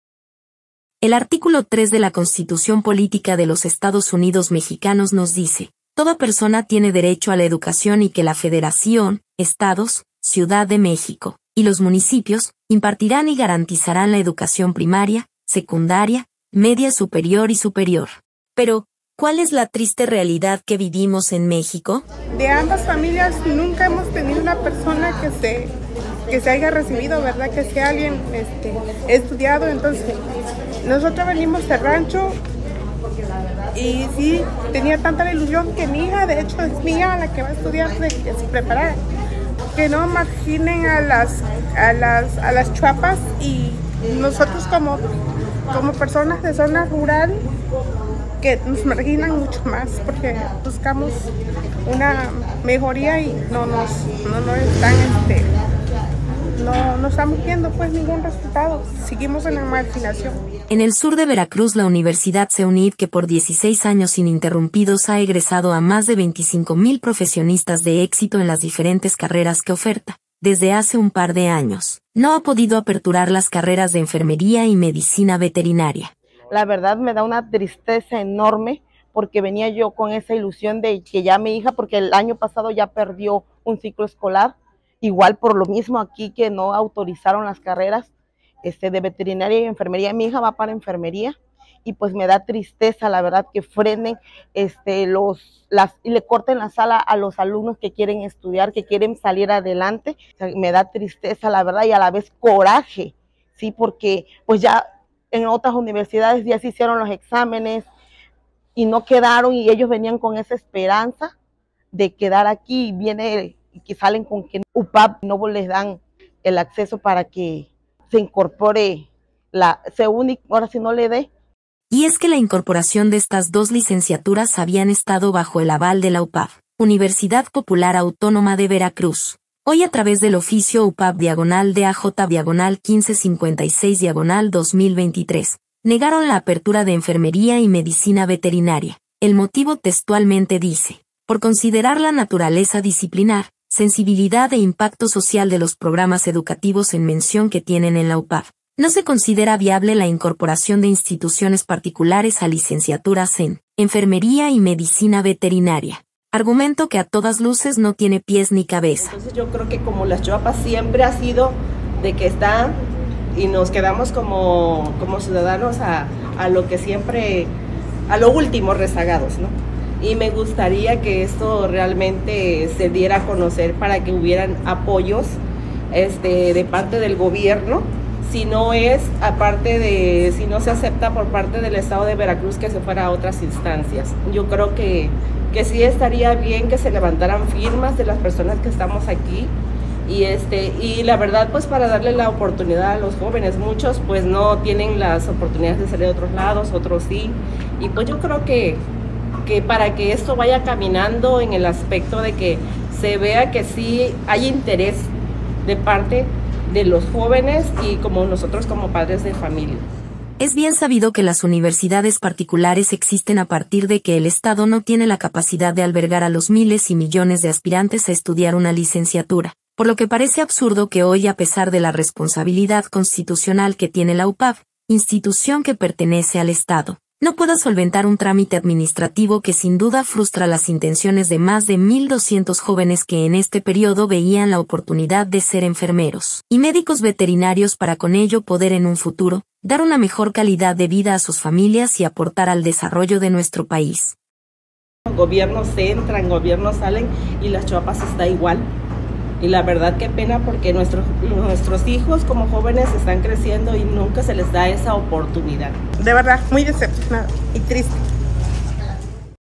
Bloquea UPAV a CEUNIV
➡️ Bloquea UPAV al CEUNIV en Las Choapas

En el sur de Veracruz el Centro Educativo Universitario Siglo XXI (CEUNIV) tiene 16 años ininterrumpidos egresando a más de 25 mil profesionistas de éxito en las diferentes carreras que oferta, desde hace un par de años; no ha podido abrir las carreras de Enfermería y Medicina Veterinaria ya el la rectoría de UPAV, del gobierno de Veracruz, bloquea el registro.